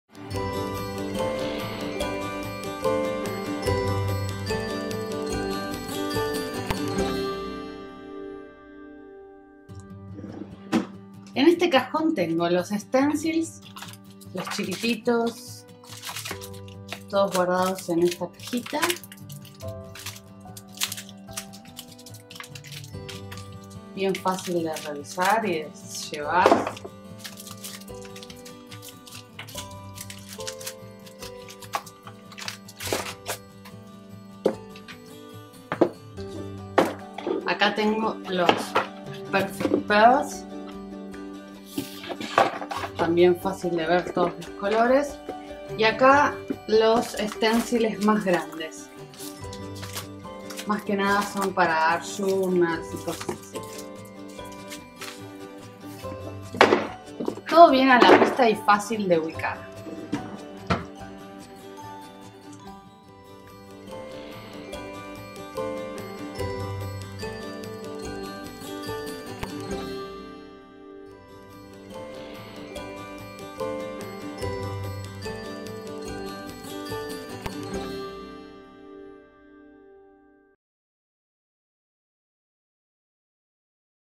Craftroom Interactivo 360 - Cajón Stencils y Perfect Pearls
Este video muestra el Cajón donde guardo los stencils y los Perfect Pearls en mi Craftroom.